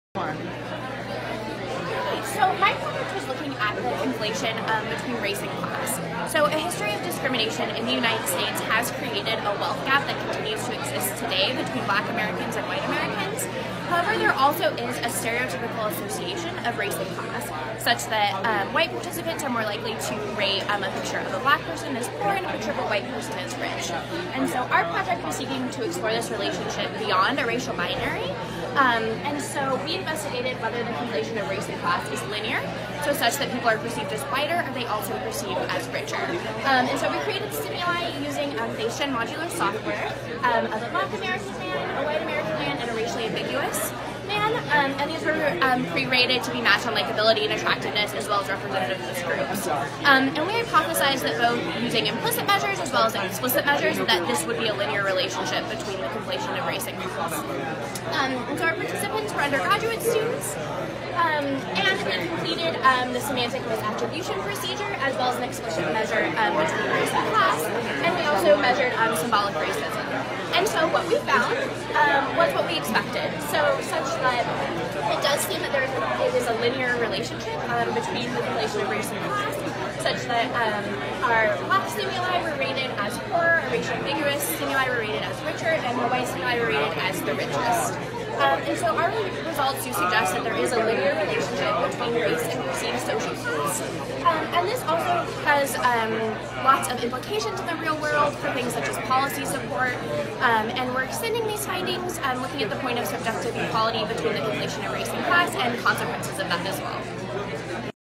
Sharla Biefeld - Race = Class?
Poster Presentation at SPSSI Summer Conference 2019. "Race = Class? Investigating Psychological
Entanglements between Race and Class"
Sharla Biefeld, University of Kentucky, Jazmin Brown-Iannuzzi,
University of Kentucky